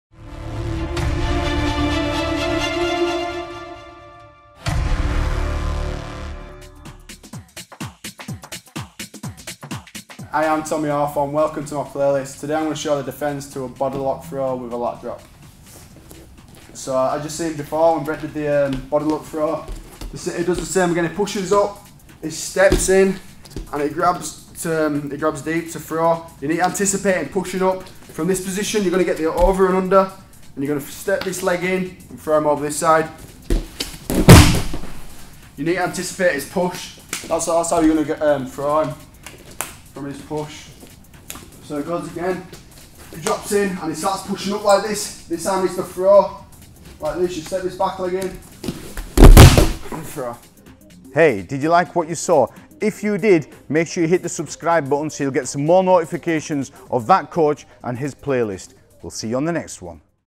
WRESTLING - Tommy Hawthorn - Body Lock Throw Defence with Lateral Drop
In this tutorial Brett demonstrates the defense to a body lock throw.

Click his playlist for more tutorial vids.